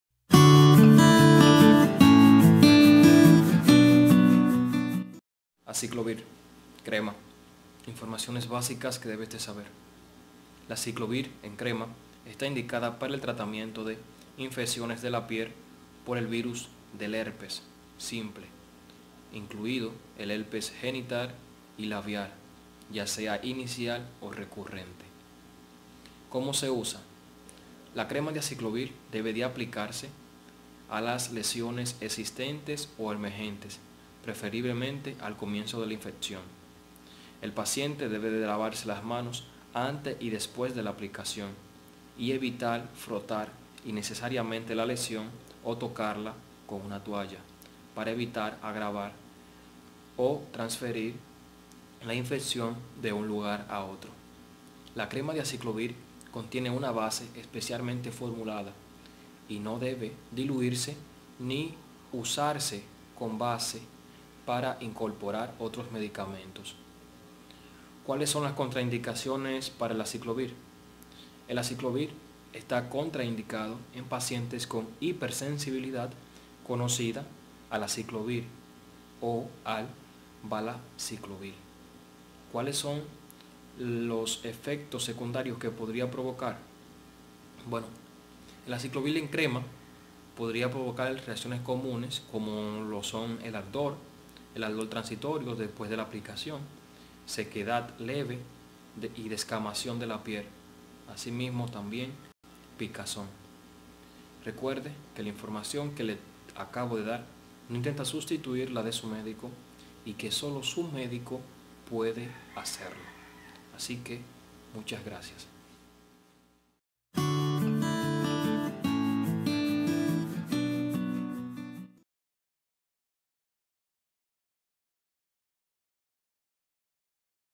ACICLOVIR CREMA: para que sirve el Aciclovir en crema| Y MAS!! 💊 🔴 Herpes
GRACIAS!! Esperamos que el Vídeo consejo sea de gran Ayuda para ti. 💊. La crema de aciclovir se usa para tratar el herpes labial (ampollas de la fiebre causadas por un virus llamado herpes simple) en la cara o los labios. El ungüento de aciclovir se usa para tratar los primeros brotes del herpes genital (infección causada por el virus herpes que esporádicamente provoca lesiones en los genitales y el recto), y para tratar ciertos tipos de lesiones causadas por el virus herpes simple en las personas con sistemas inmunitarios débiles. Pertenece a una clase de medicamentos antivíricos llamados análogos de los nucleósidos sintéticos. Funciona al detener la propagación del herpes en el cuerpo.